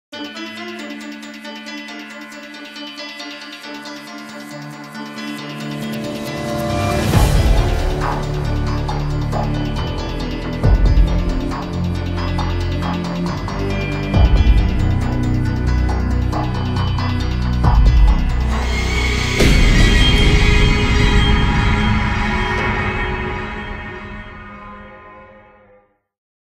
Saw TV Series (Intro)
Me and Maslinmedia are slowly managing to pu together a tv series for the saw franchise, coming soon...